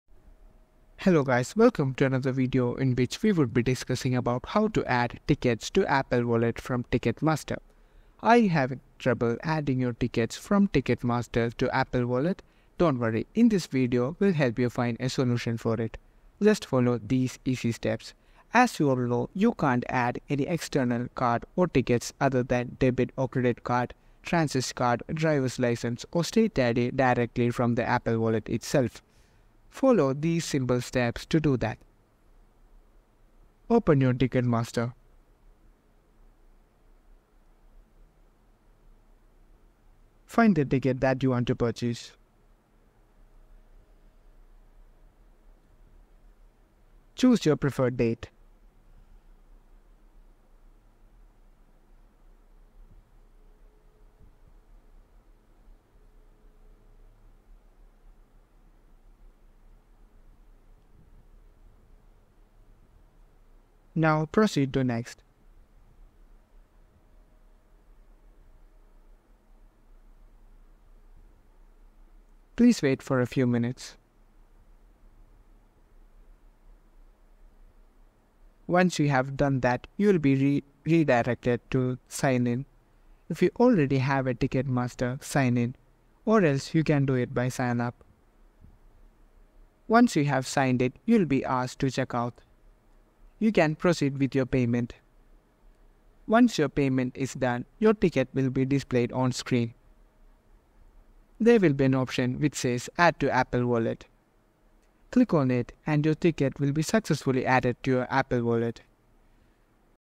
How To Add Tickets To Apple Wallet From Ticketmaster
In this tutorial, learn how to easily add tickets purchased from Ticketmaster to your Apple Wallet. Follow the step-by-step guide to ensure your tickets are easily accessible on your iPhone.